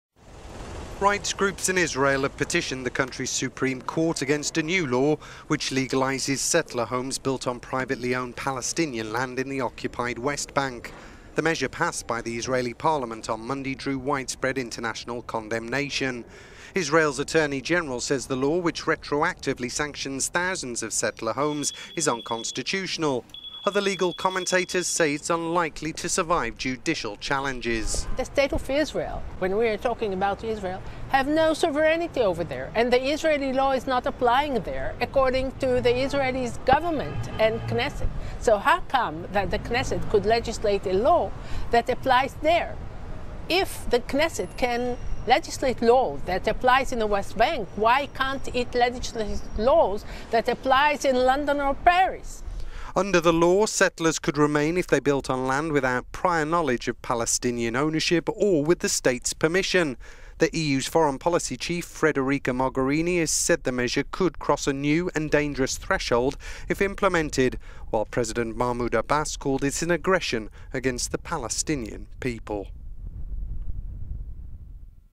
Rights groups in challenge controversial new Israeli settler law
Rights groups in Israel have petitioned the country's Supreme Court against a new law which legalises settler homes built on privately owned Palestinian land in the occupied West Bank.

The measure passed by the Israeli parliament on Monday drew widespread international condemnation.

Israel's attorney-general says the law, which retroactively sanctions thousands of settler homes, is unconstitutional.

Other legal commentators say it is unlikely to survive judicial challenges.